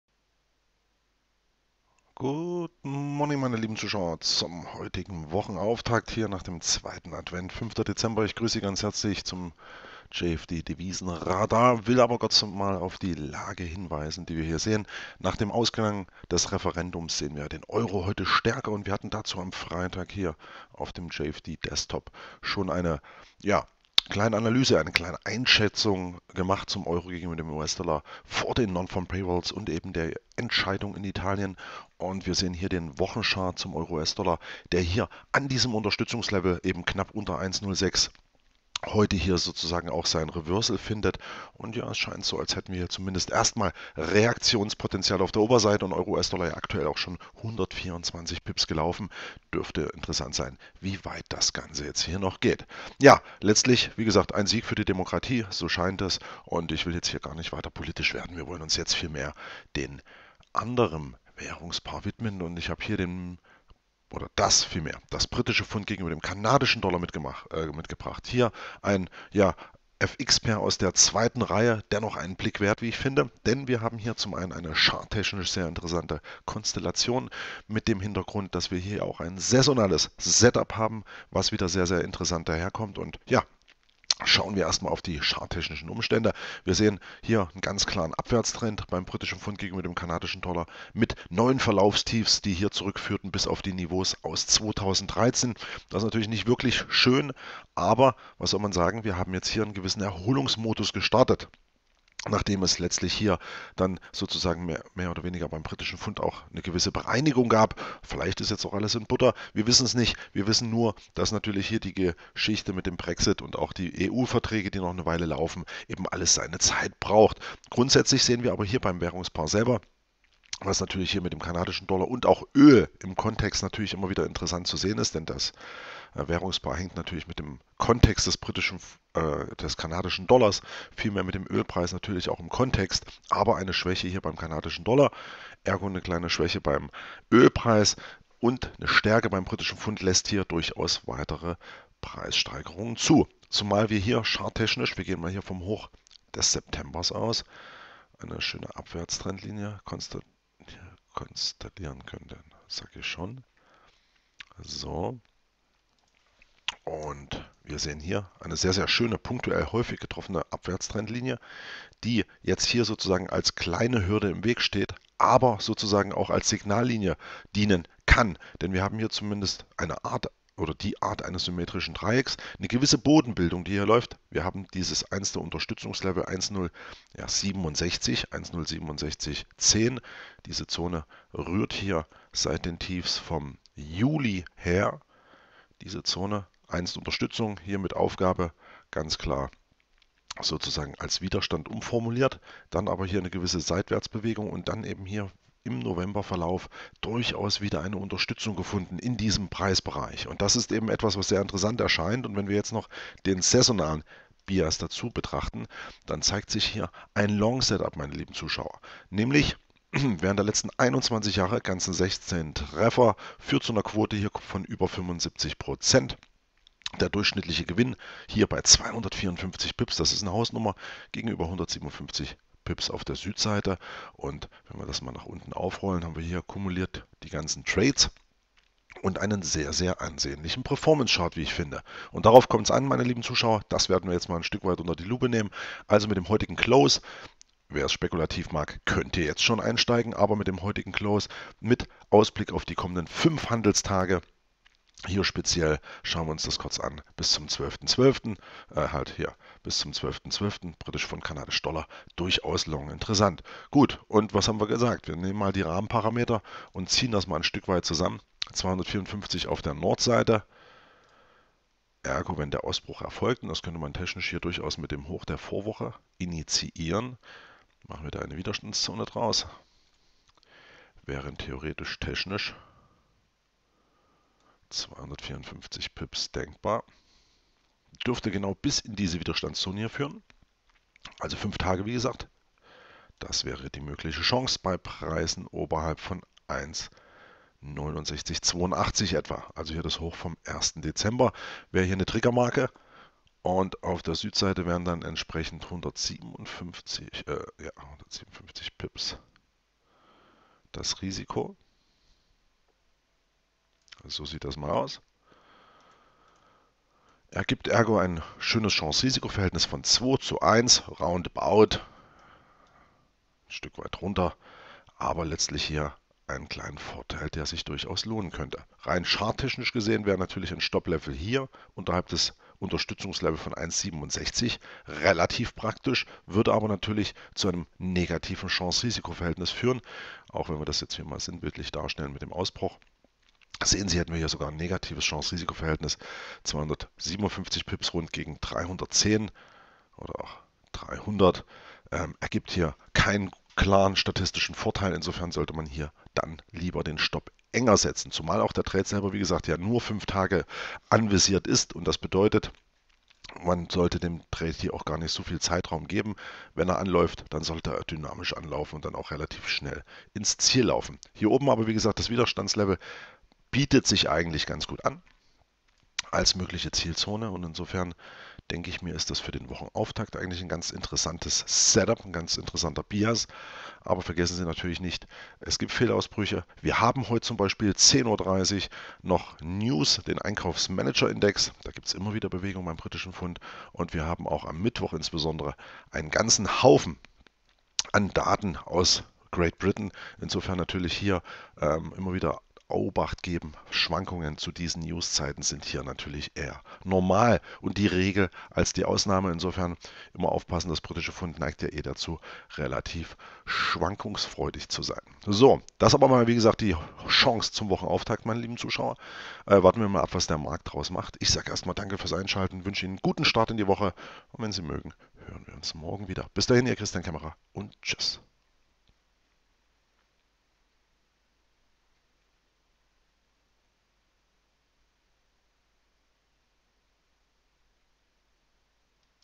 JFD Devisenradar: GBP/CAD mit Long-Chance für 5 Tage
Zum heutigen Wochenauftakt findet sich eine interessante Konstellation beim GBP/CAD. Hierbei lässt sich eine saisonale wie charttechnisch interessante Ausgangsbasis lokalisieren, welche Chance aufzeigt.